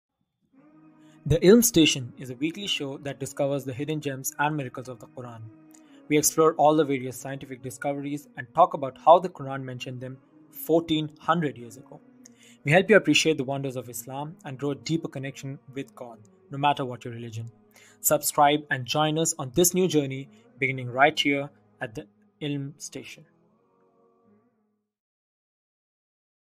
Trailer - THE ILM STATION
'The Ilm Station' is a new podcast for the youth. We talk about the 'Miracles of the Quran', 'Facing the difficulties of life' the Islamic way and many more. We also invite special guests every month to inspire and motivate us.